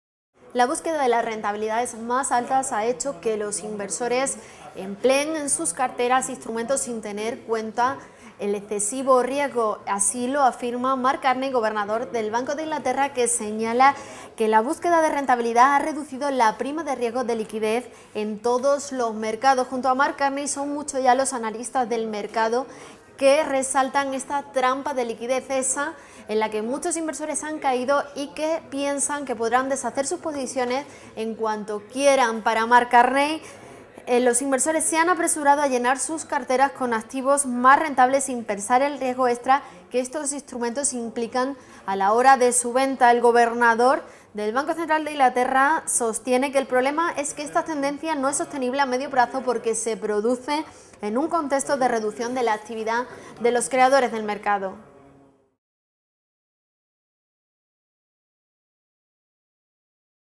¿Cuál es el riesgo sistémico que trae de cabeza a los inversores?
Los inversores se han apresurado a llenar sus carteras con los activos más rentables, sin pensar en los riesgos asociados a los mismos.

No son pocos los analistas que advierten de la trampa de la liquidez. ¿En qué consiste?